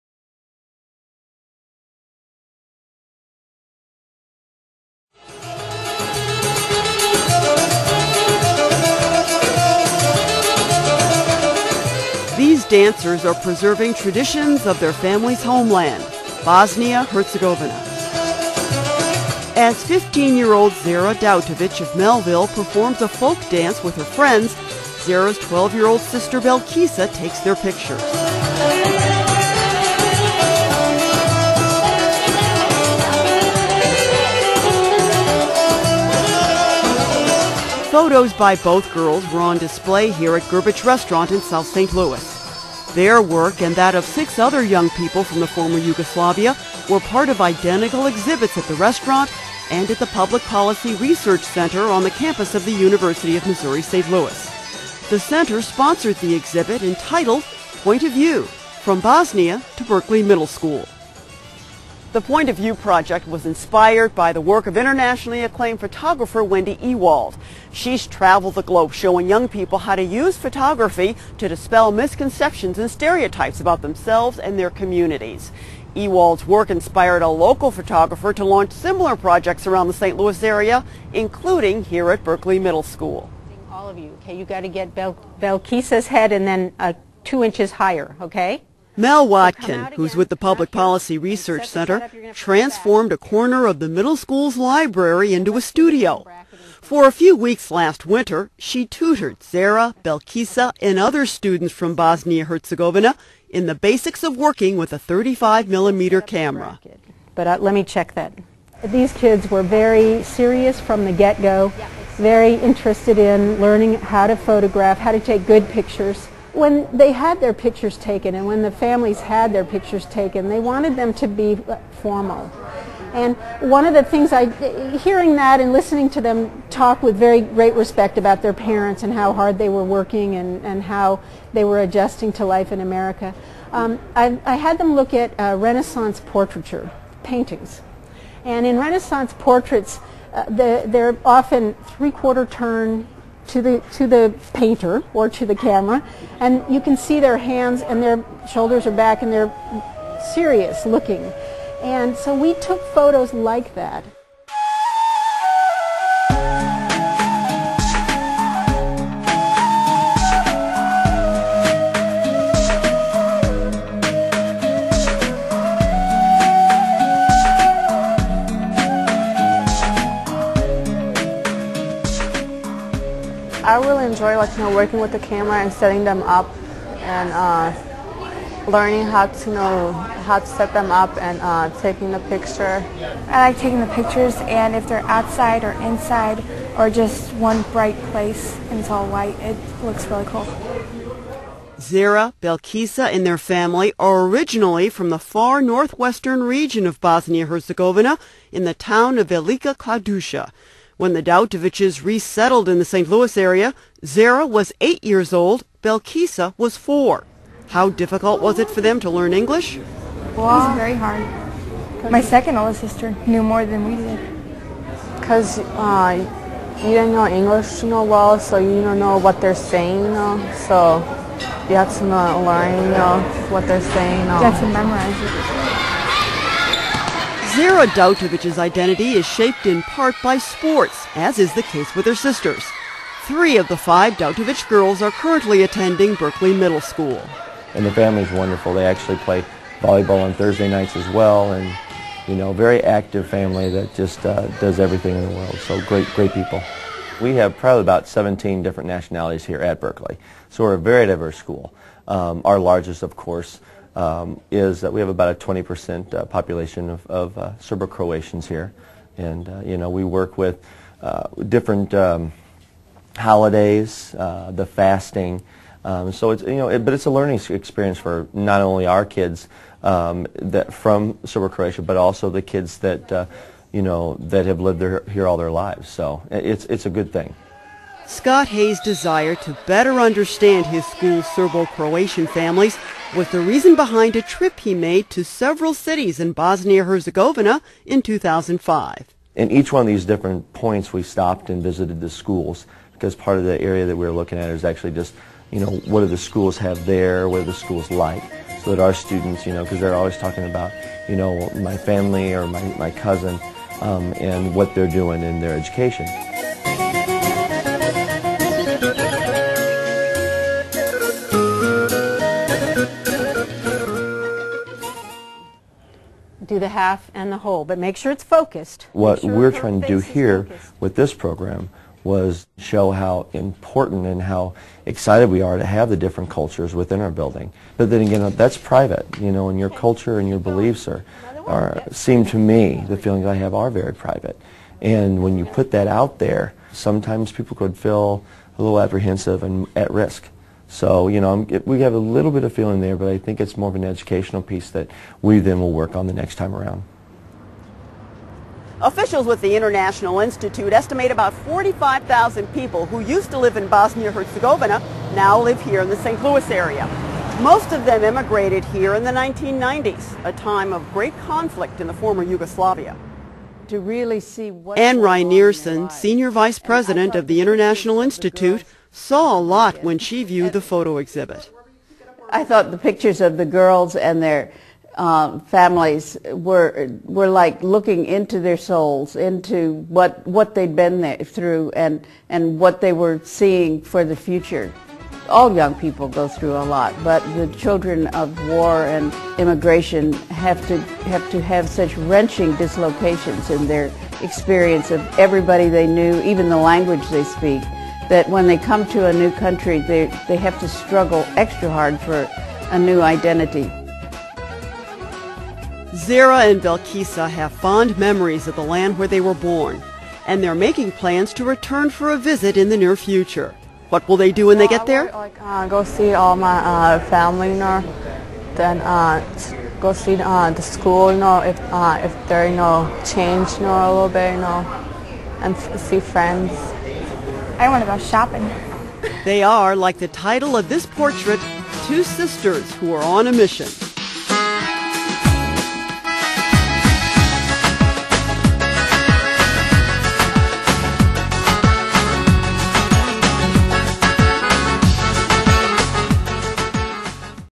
Bosnian Photos | Living St. Louis
Living St. Louis producer Ruth Ezell discovers how the Bosnian youth in St. Louis are capturing their culture and traditions through photography.  Photographer Mel Watkin teaches the students at Buerkle Middle School how to use the camera to create timeless photographs.